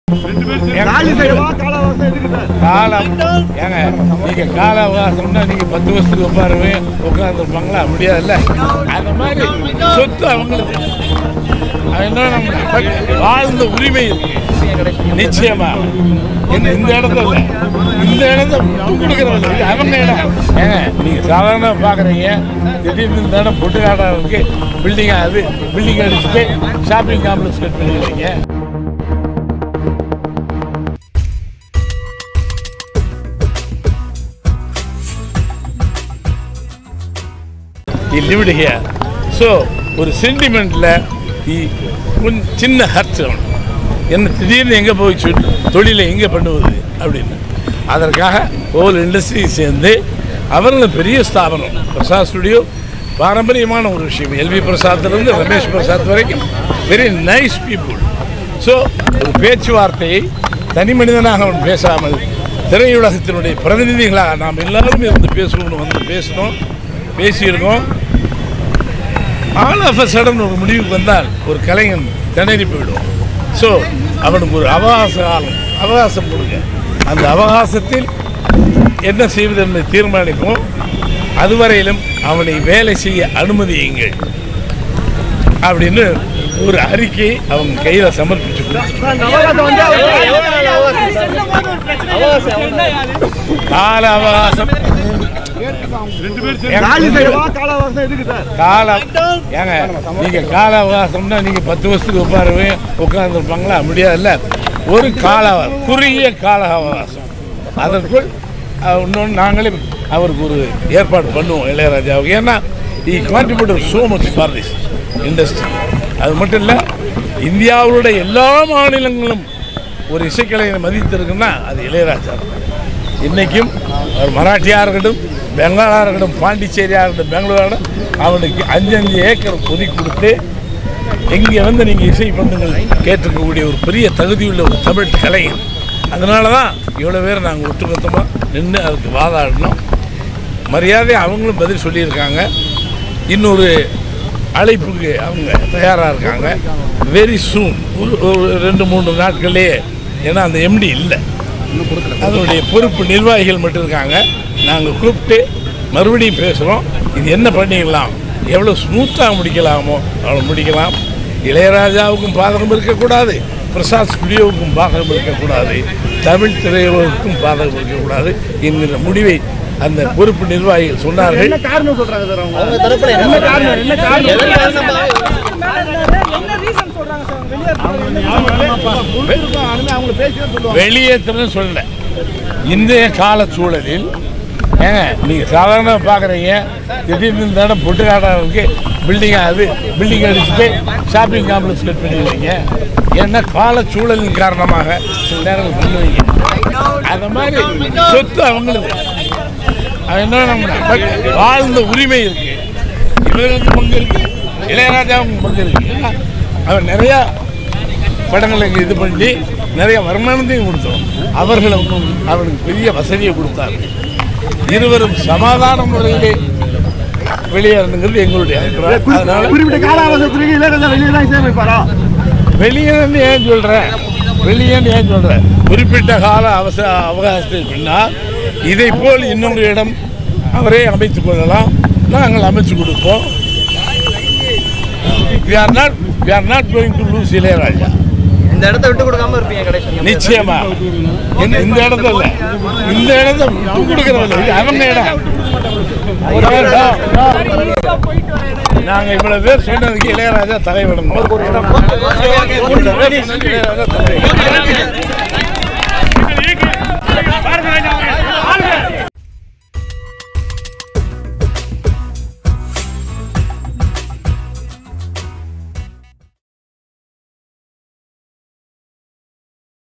இளையராஜாவுக்கு அவகாசம் கொடுங்கள் | கொந்தளித்த பாரதி ராஜா | Ilayaraaja Issue | Bharathiraja Speech
Ilayaraja who was composing music for the past 45 years in prasad studio has been kicked out from the place recently. Ace directors and film personalities Bharathiraja Bhagyaraj seaman came in support of Ilayaraja and supported him in this issue.